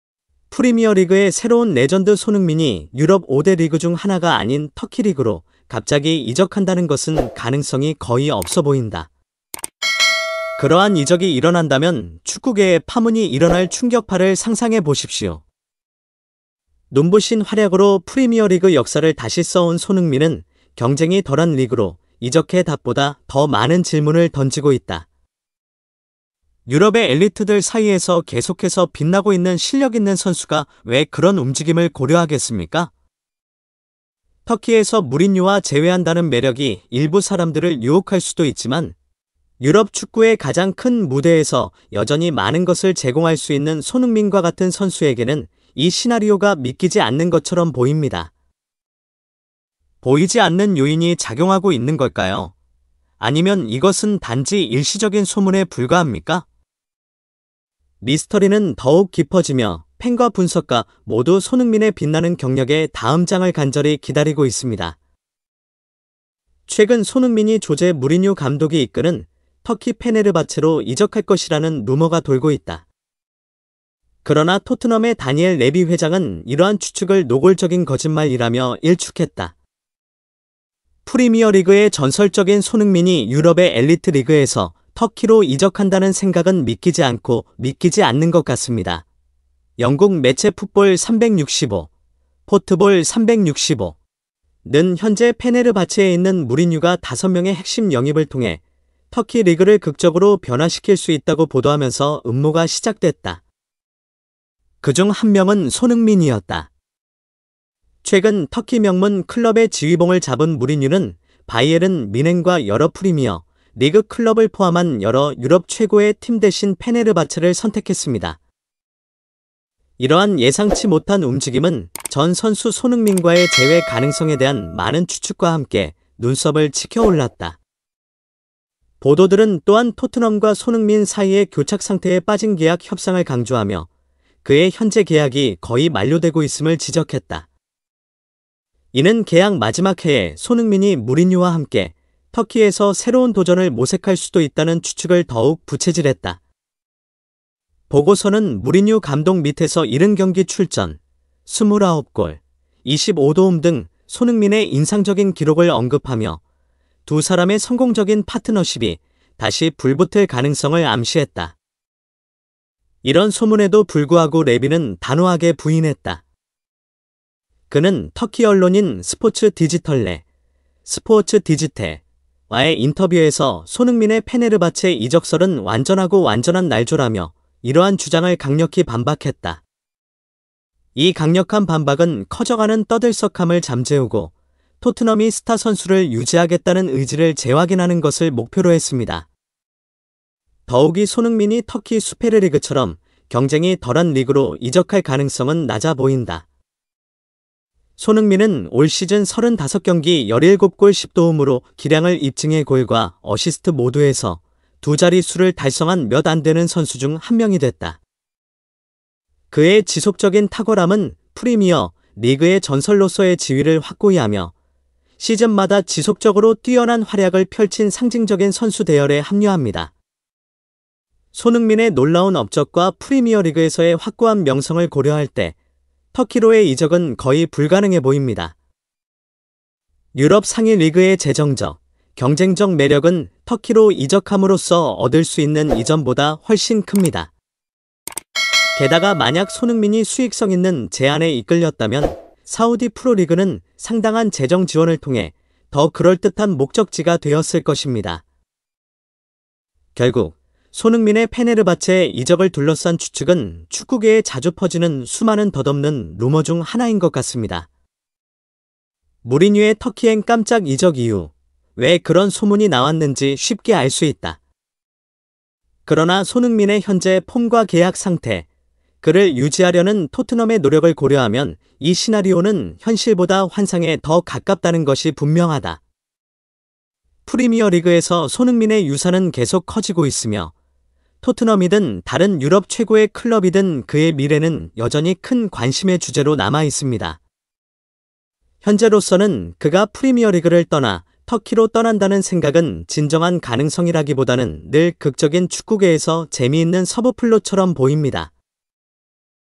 손흥민 이적 루머? 레비 회장 "SON 절대 안 팔아!"
손흥민의 페네르바체 이적 루머가 돌고 있는 가운데, 토트넘의 레비 회장이 직접 이를 부인했습니다. "새빨간 거짓말"이라고 단호히 말하며 손흥민은 절대 판매하지 않을 것이라고 강조했습니다. 손흥민의 미래에 대한 관심이 높아지고 있는 상황에서, 이번 발표는 팬들에게 큰 안심을 주고 있습니다. 자세한 내용은 아래에서 확인하세요.